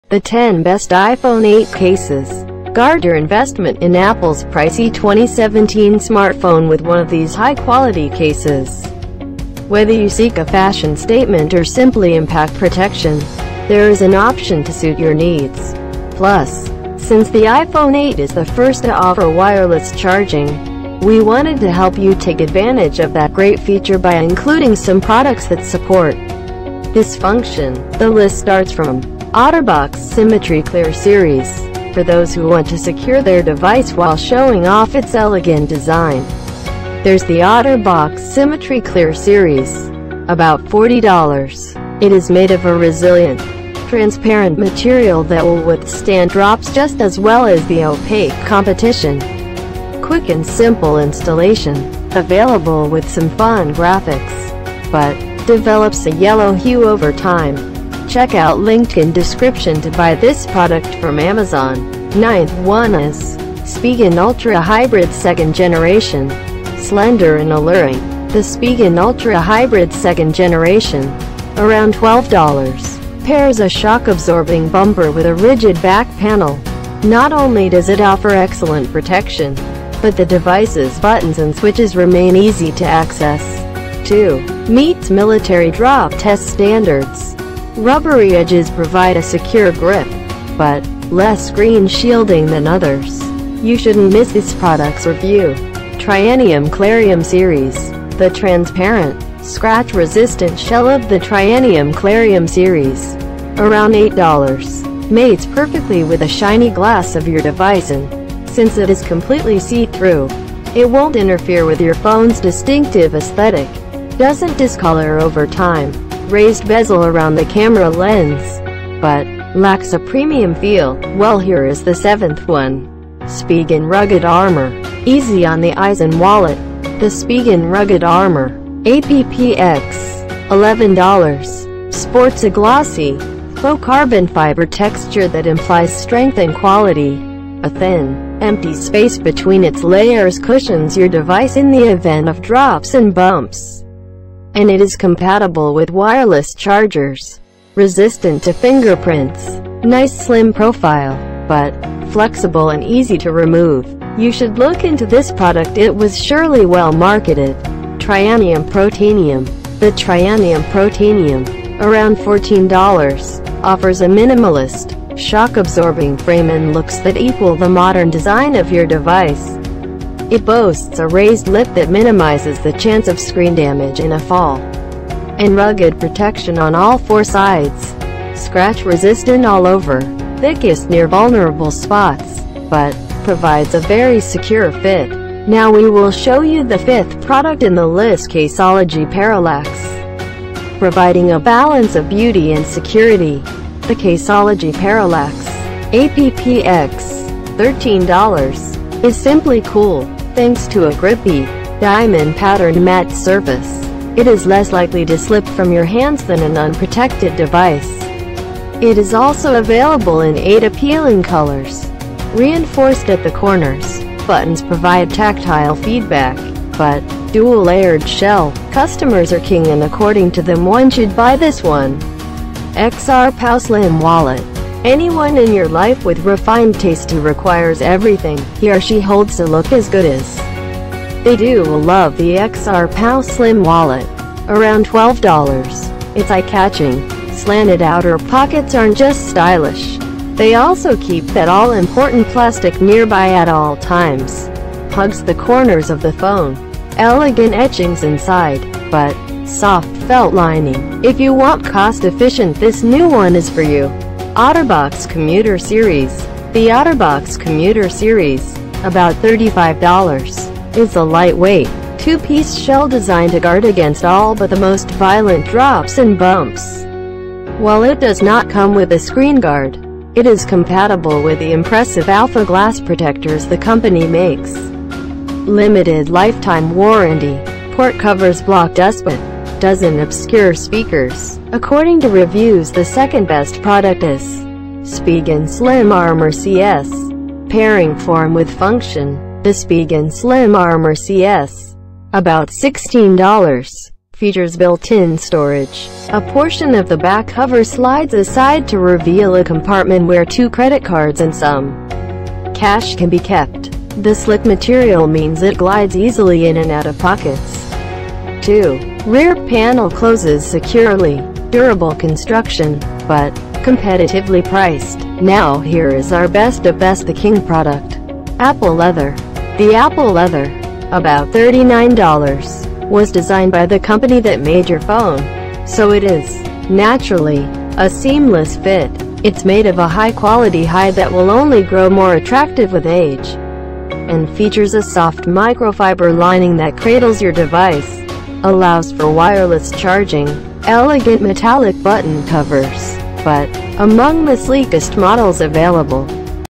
👉 The 10 Best iPhone 8 Cases 2020 (Review Guide)
After 100's of customers and editors reviews of Best iPhone 8 Cases, we have finalised these Best 10 products:

👉 1 Apple Leather
👉 2 Spigen Slim Armor CS
👉 3 OtterBox Commuter Series
👉 4 XRPow Slim Wallet
👉 5 Caseology Parallax
👉 6 Trianium Protanium
👉 7 Spigen Rugged Armor
👉 8 Trianium Clarium Series
👉 9 Spigen Ultra Hybrid 2nd Generation
👉 10 Otterbox Symmetry Clear Series